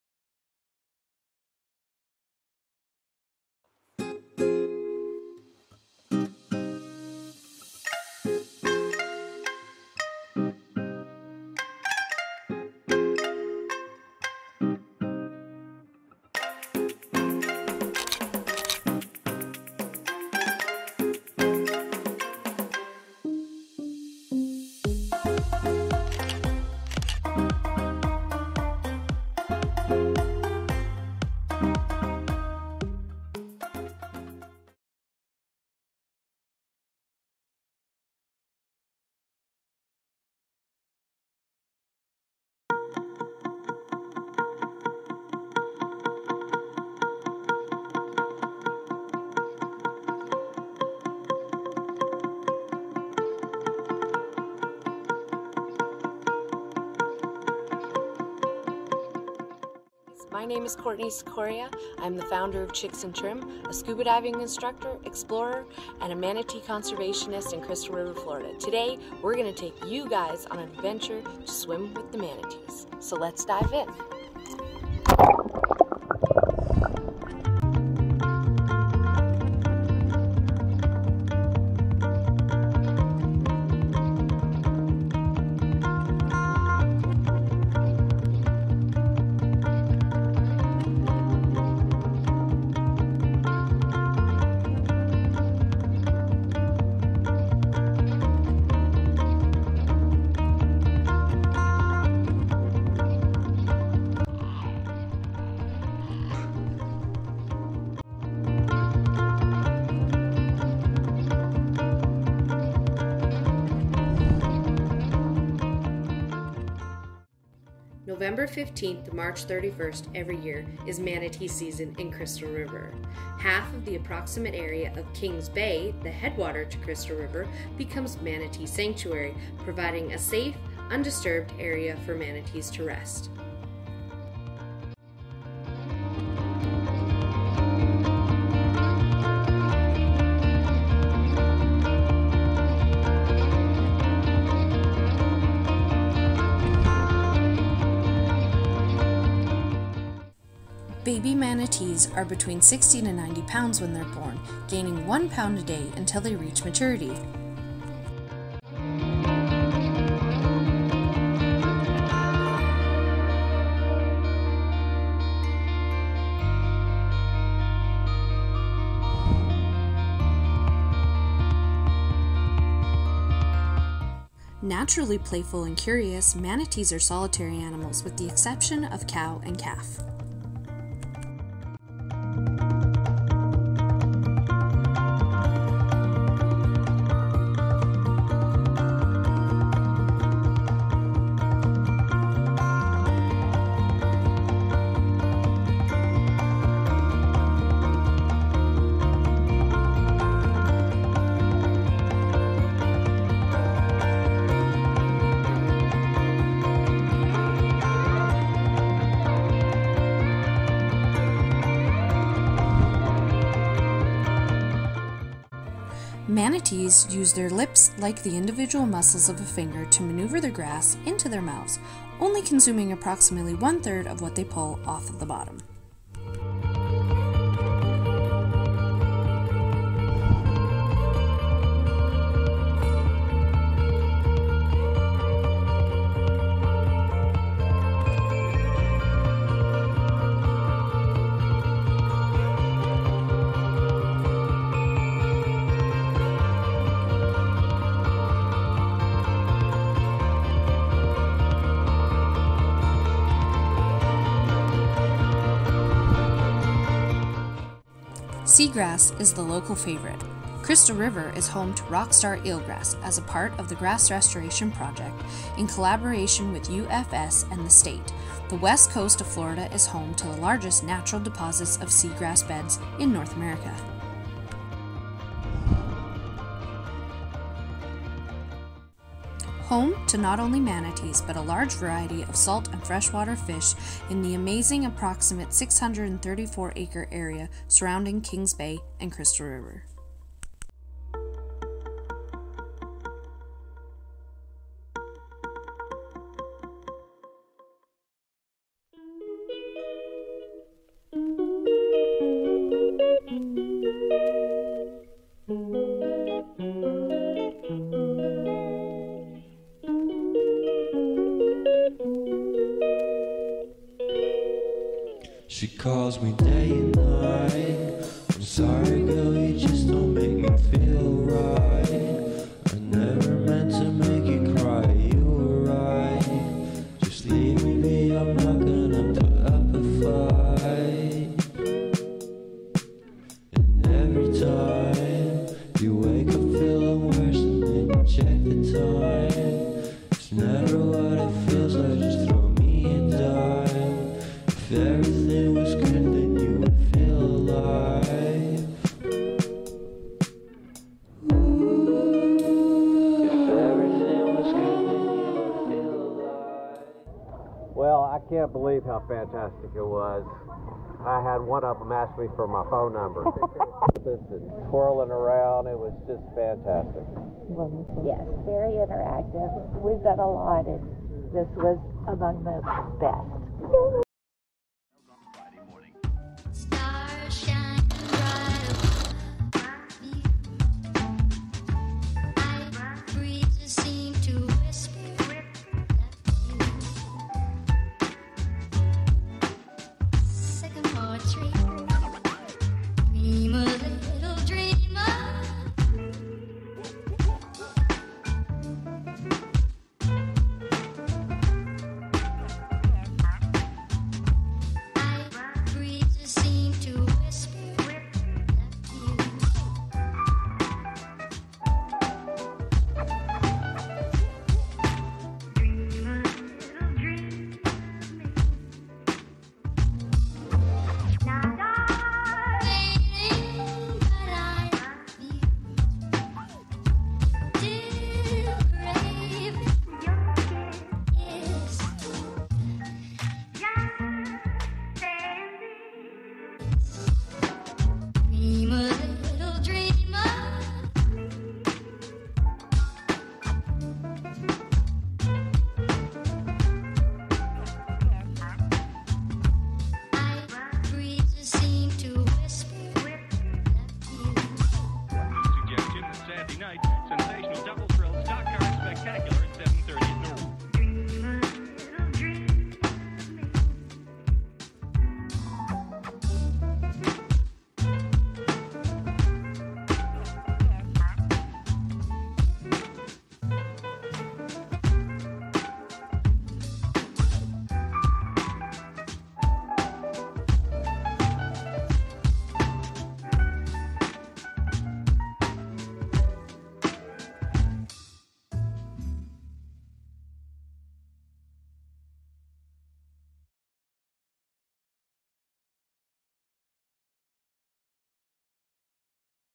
Rockwater Explorers - Short Ep.1 Manatees
We take you on an underwater adventure with Florida's favorite local residents, The West Indian Manatee.